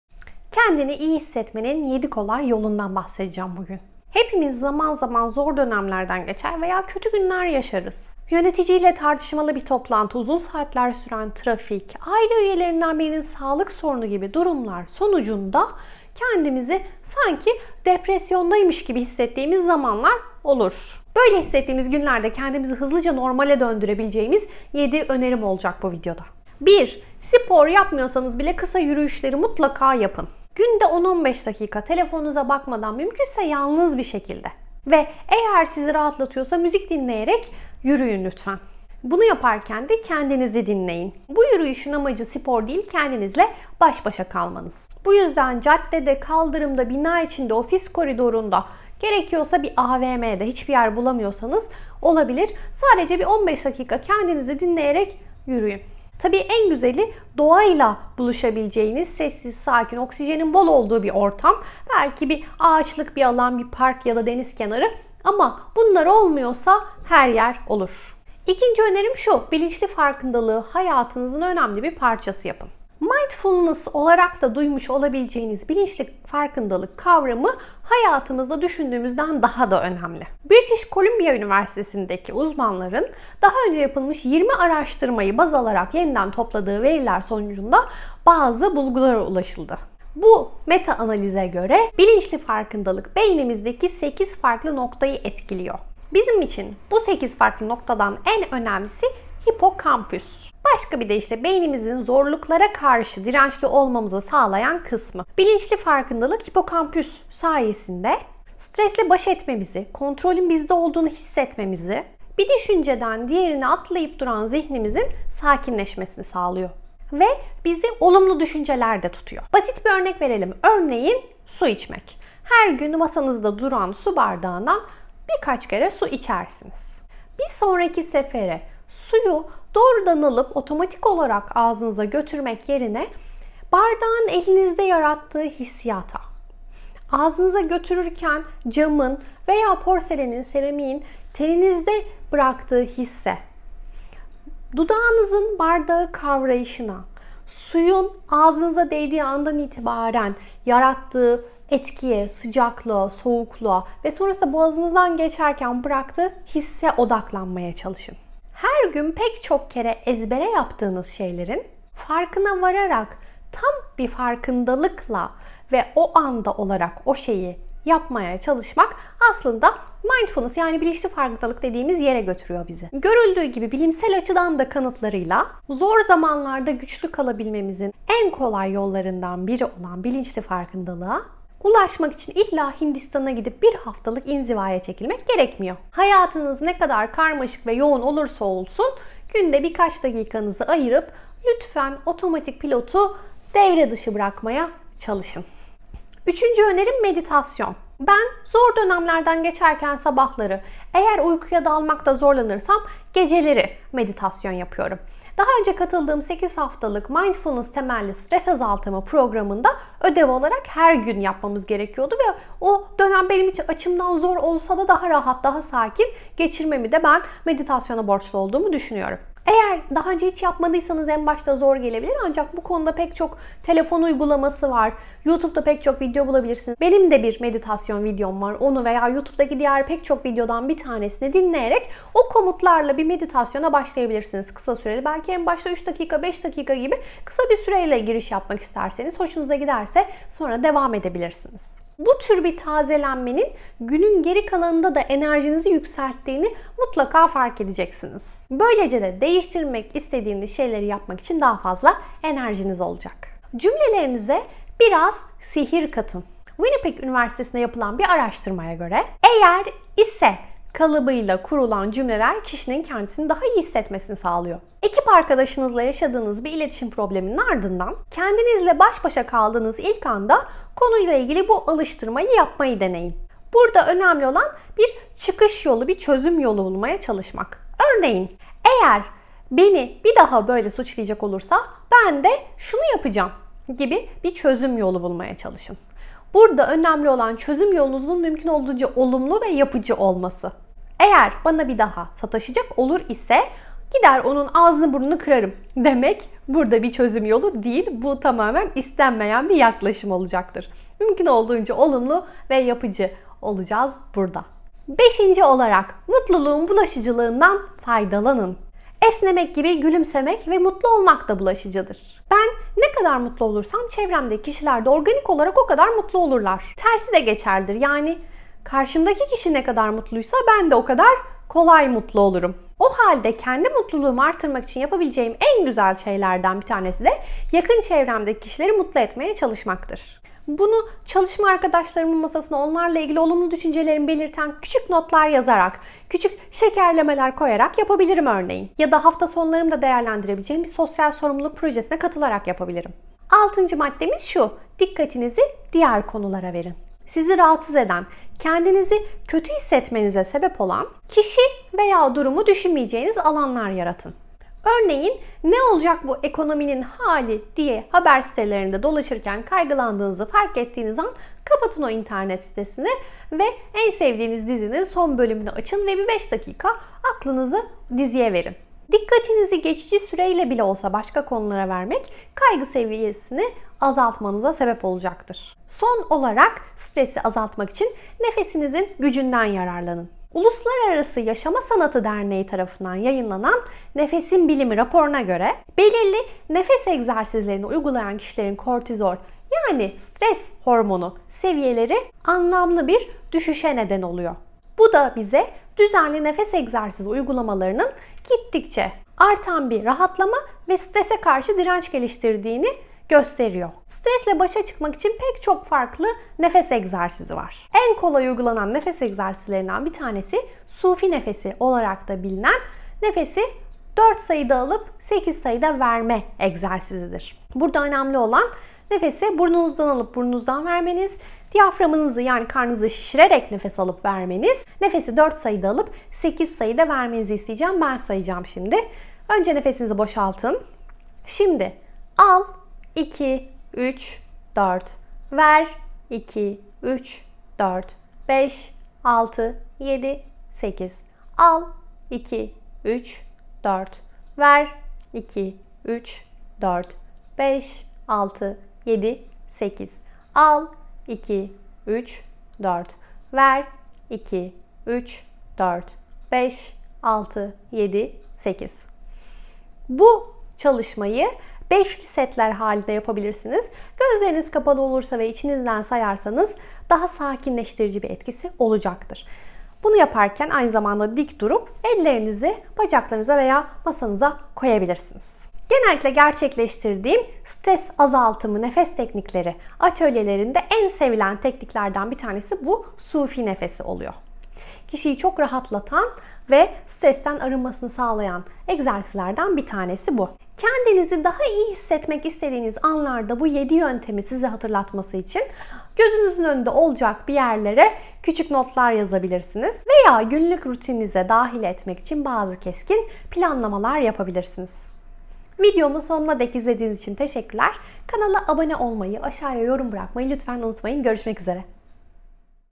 Daha İyi Hissetmenin 7 Yolu | Mutluluk Alışkanlıkları
Hepimiz zaman zaman zor dönemlerden geçer veya kötü günler yaşarız. Yöneticiyle tartışmalı bir toplantı, uzun saatler süren trafik, aile üyelerinden birinin sağlık sorunu gibi üstüste gelen kötü durumlar sonrasında depresyonun eşiğinde hissettiğimiz günlerde bizi hızlıca normale döndürebilecek birkaç öneride bulunmak için bu videoyu hazırladım.

Profesyonel İlişki Ağı Nasıl Kurulur? | Networking Yapmak